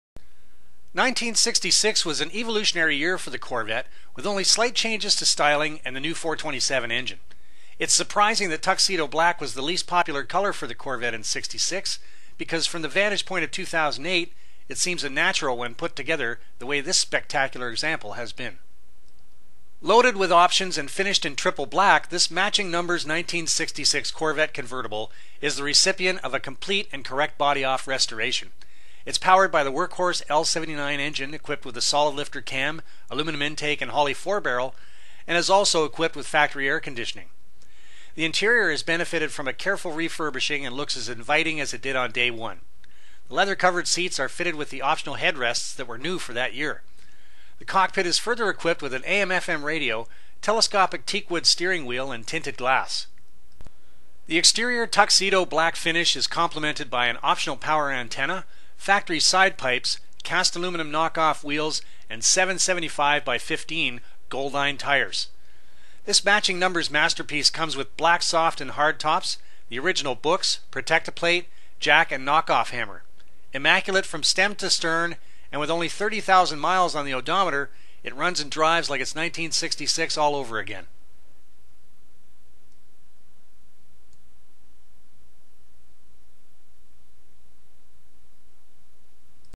1966 Chevrolet Corvette Convertible 427 4-Speed A/C
Lot S134
Loaded with options and finished in triple black, this matching-numbers convertible is the recipient of a complete and correct body-off restoration. It's powered by the workhorse L79 engine equipped with a solid lifter cam, aluminum intake and Holley four-barrel. As noted elsewhere in the catalog, it's unusual to find a 4-speed convertible of this vintage equipped with factory air conditioning, but this car correctly has both.

This matching-numbers masterpiece comes with black soft- and hard tops, the original books, Protect-O-Plate, jack and knock-off hammer. Immaculate from stem to stern and with only 30,000 miles on the clock, it runs and drives like it's 1966 all over again.

Mecum High Performance Auction Kissimmee FL Jan. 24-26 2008